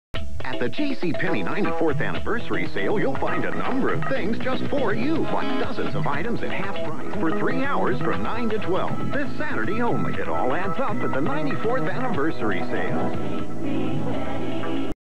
1996 JCPenney commercial
Promoting their 94th Anniversary Sale